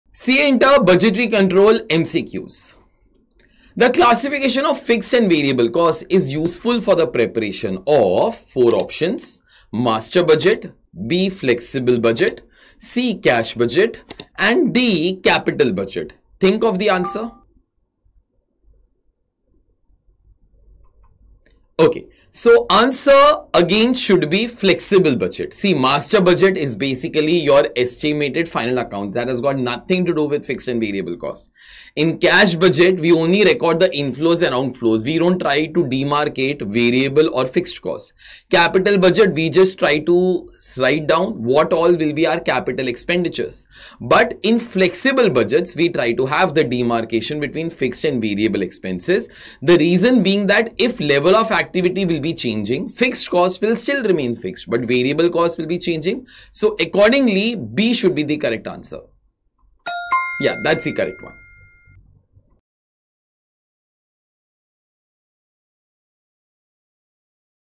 Mastering Budgetary Control - MCQ of CA INTER || MCQ-2|| Classification | Costing World
🚀 This is MCQ# 2 from CA INTER - Budgetary Control. After Sir presents the 4 options, hit pause, and drop your answer with the logic in the comments section! 🤔💡